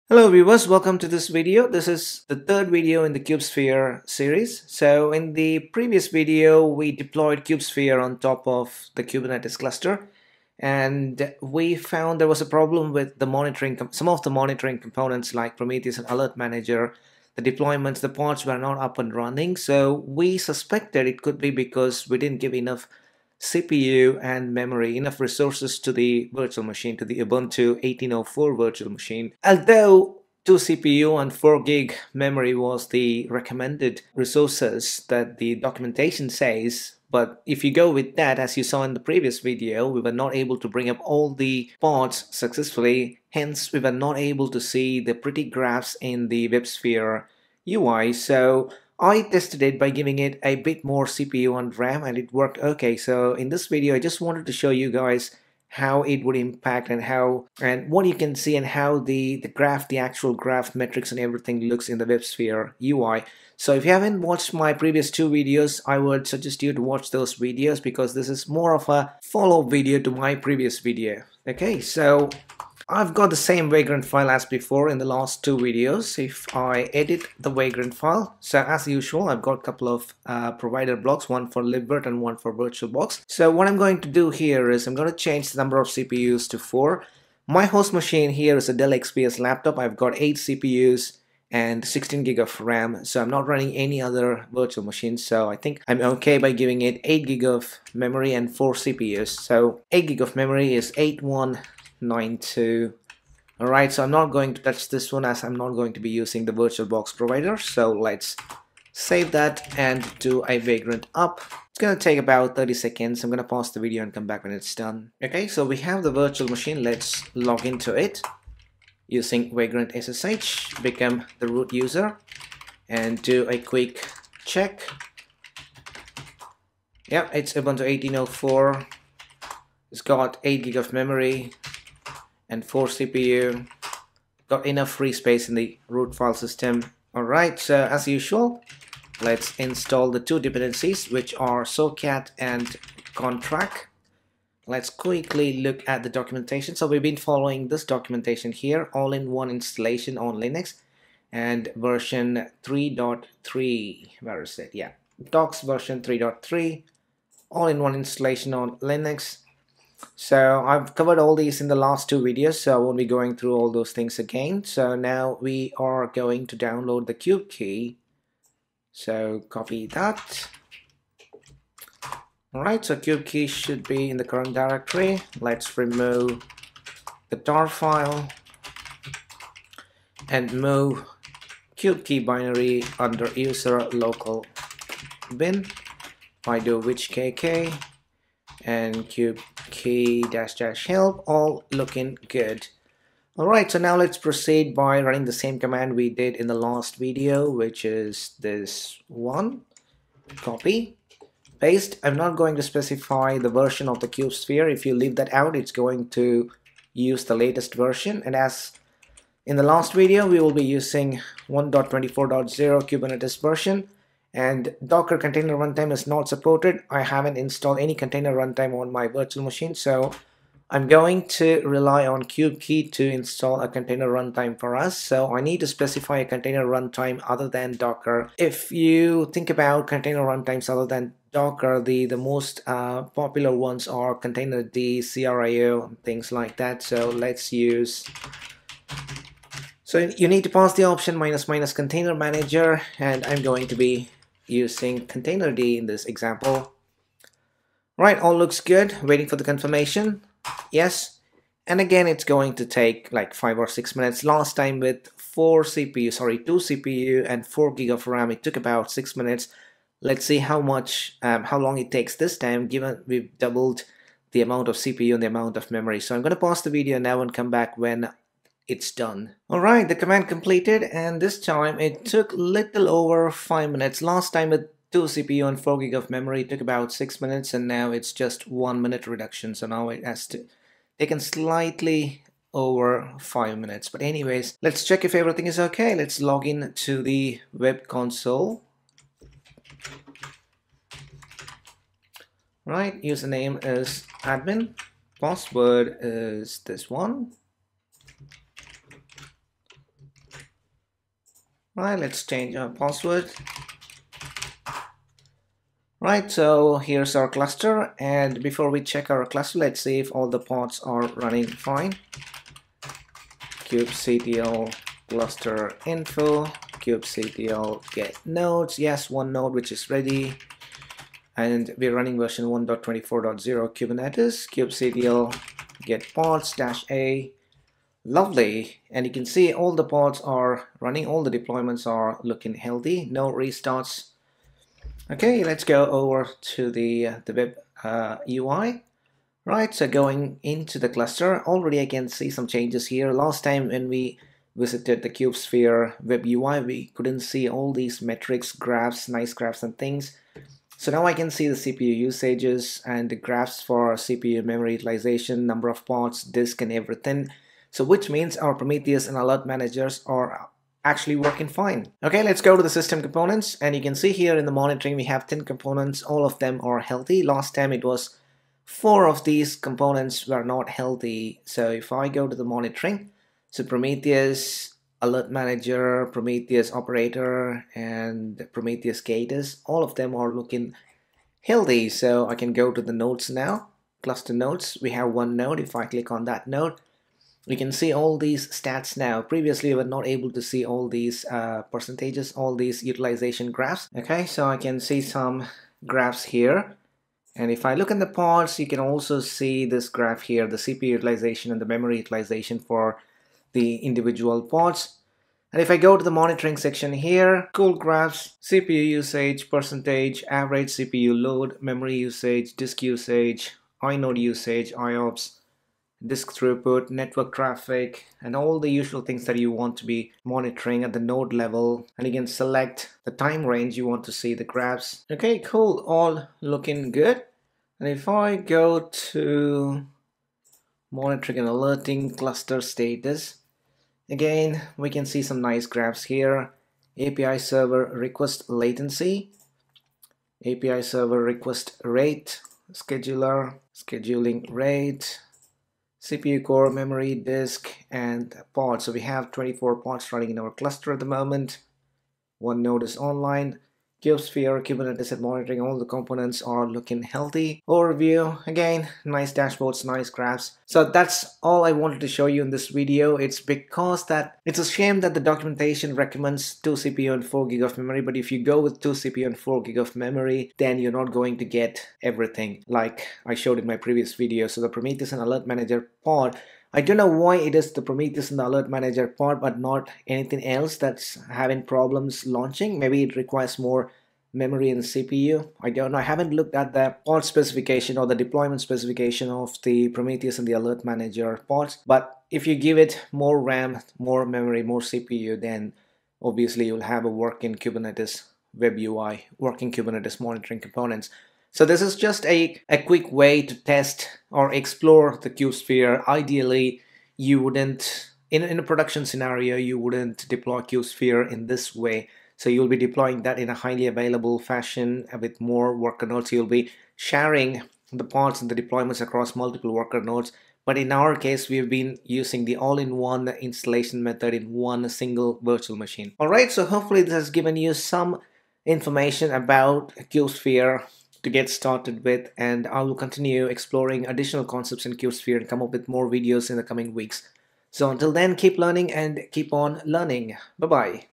[ Kube 113.3 ] Learn Kubesphere | Installing & Exploring Kubesphere Web UI | Part 2
In this video I will show you how to provision a kubernetes cluster and kubesphere using KubeKey. We will also explore the Kubesphere Web UI.

KubeSphere is a distributed operating system for cloud-native application management, using Kubernetes as its kernel. KubeSphere is also a multi-tenant container platform.